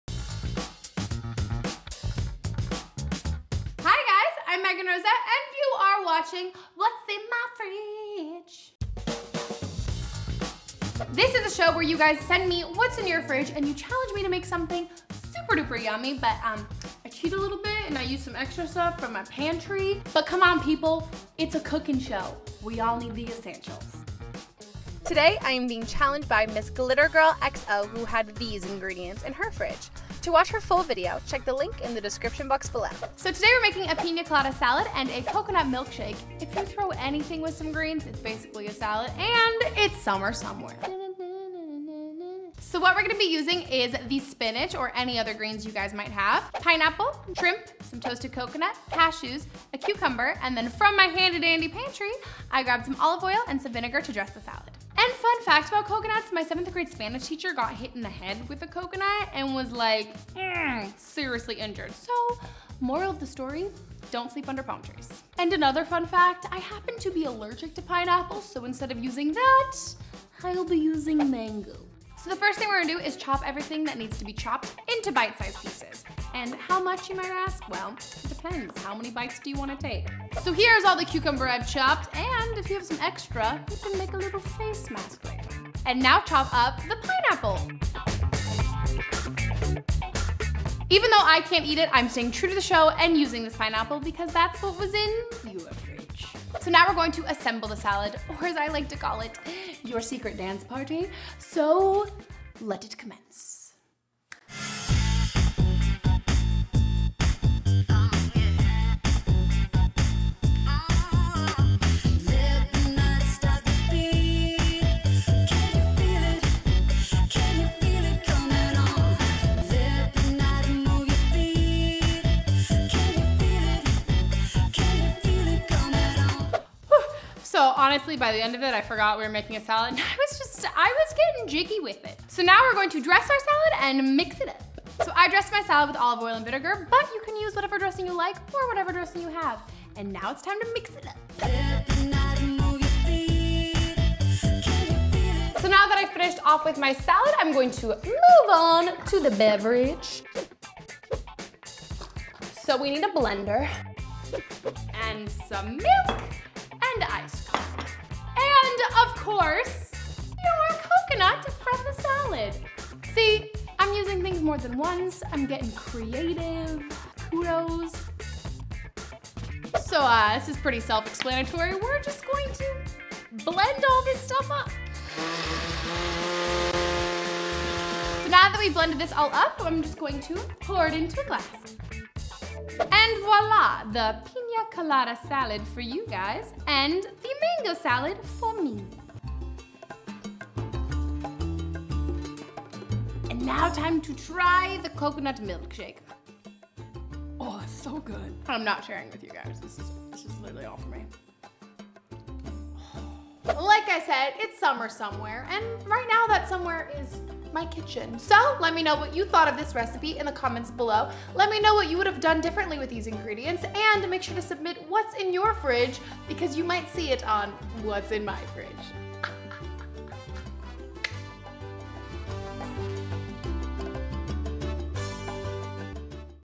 Pina Colada with MeghanRosette - What's In My Fridge?
Chef Meghan Rosette raids our fridge to see if she can whip up something yummy (like Pina Colada salad!) out of the random things from user submitted videos!

CREDITS
Instagram.com/MeghanRosette